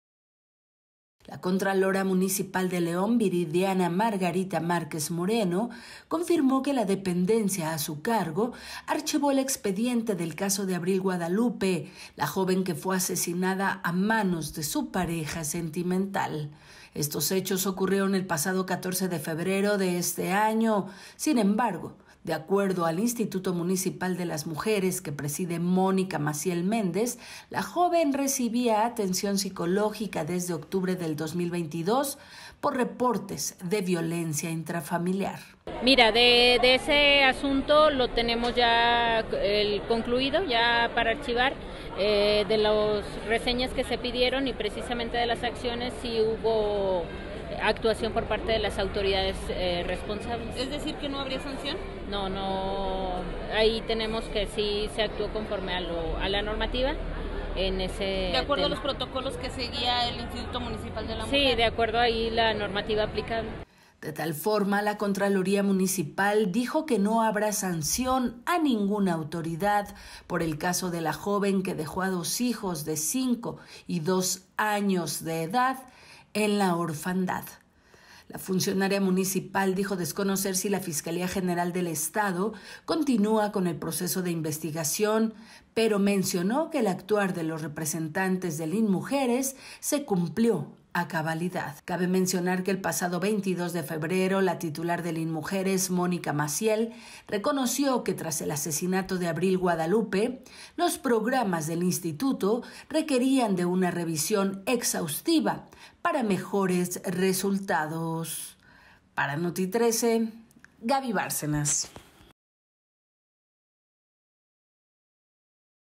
Contraloría municipal archiva caso de Abril Guadalupe
VideoNota🎥🔴 | Contraloría municipal de León archivó el caso de Abril Guadalupe la joven que fue asesinada a manos de su pareja el pasado mes de febrero.